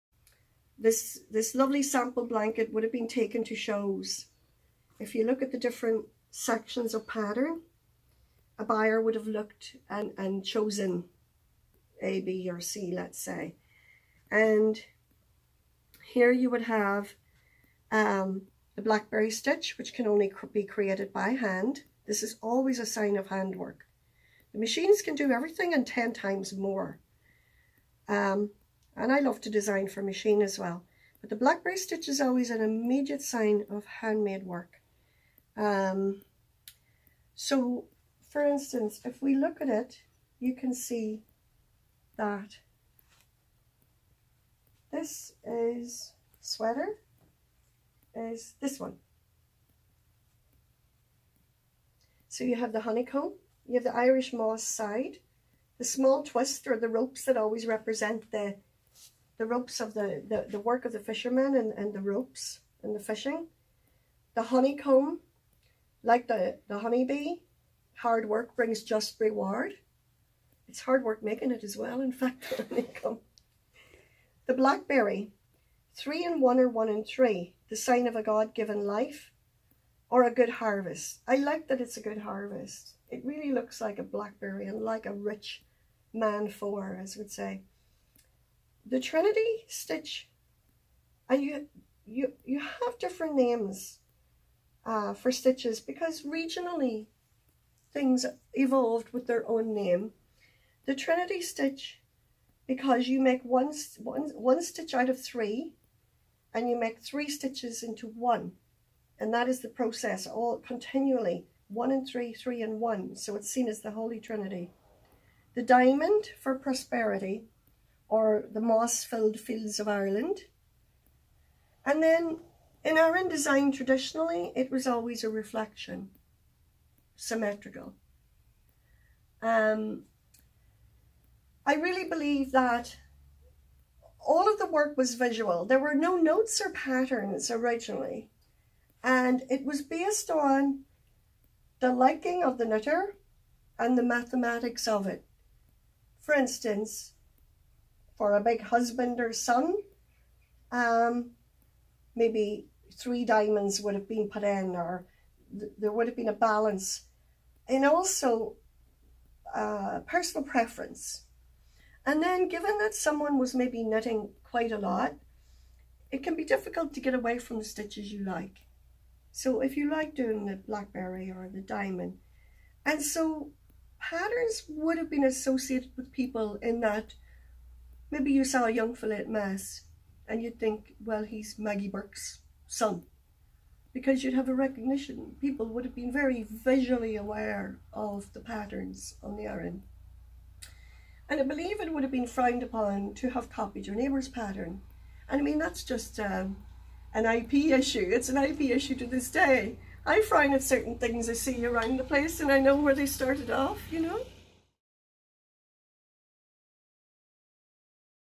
In The Closet: Edel Macbride
This week on ‘In The Closet’ our Creative Director, Bernadette Casey, learns the fascinating history behind the Aran jumper with designer Edel Macbride in her beautiful Donegal workroom.

MacBride grew up with the tradition of Aran knitting at home in Donegal and starting knitting for her younger siblings and progressed to study fashion at Limerick School of Art in the 1980's.

An Aran knit coat soon brought her international attention and numerous awards. Her label "Edel MacBride" is hand-made in Ireland and sold around the world and her business "Knitfield" runs workshops to inspire others to take up yarn craft and a dedication to design and quality of workmanship is at the heart of "Knitfield."